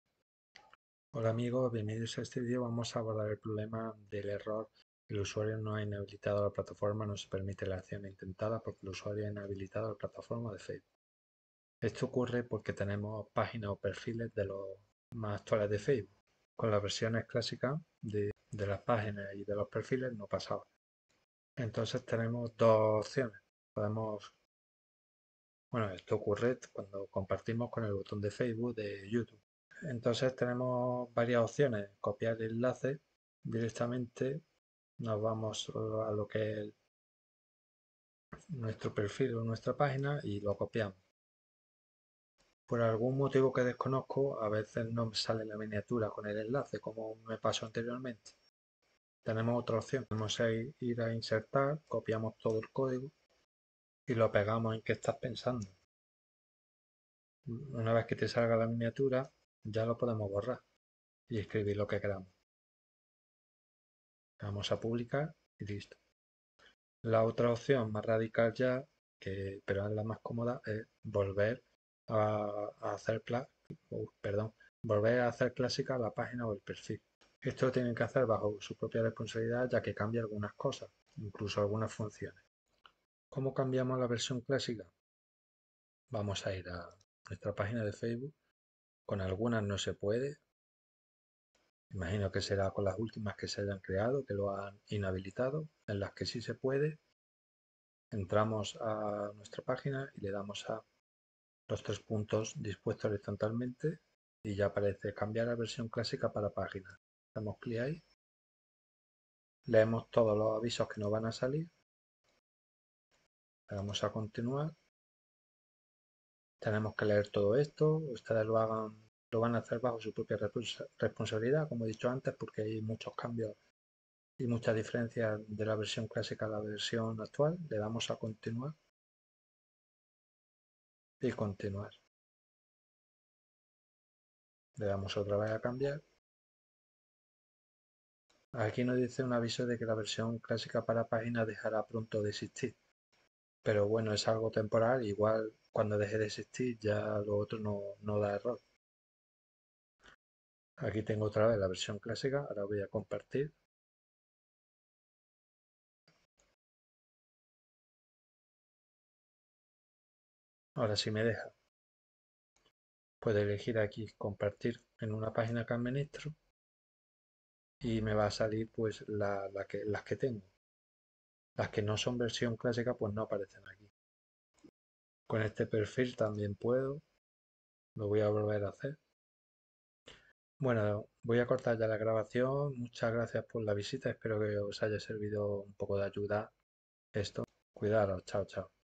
Solución al Error de no poder compartir vídeo de YouTube con página o perfil de Facebook
Solución al Error El usuario ha inhabilitado la plataforma: No se permite la acción intentada, porque el usuario ha inhabilitado la plataforma de Facebook.